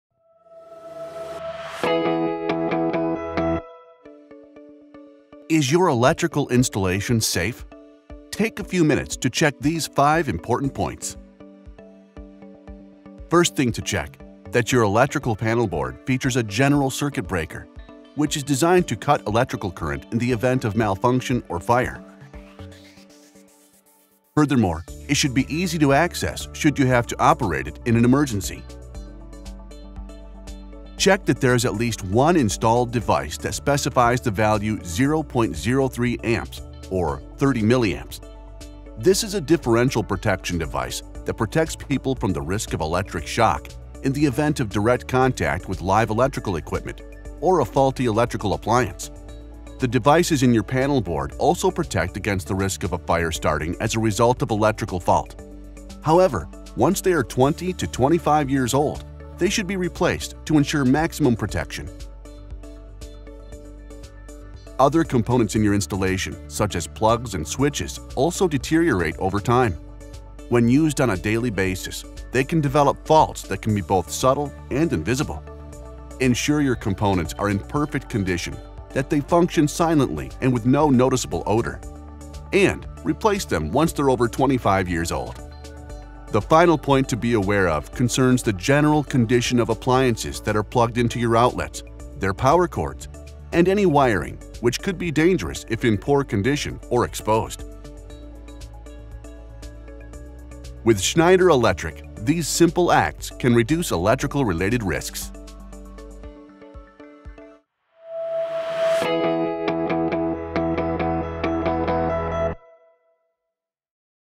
Electrical Safety Essentials
Examine five main areas crucial to the safety of your installation.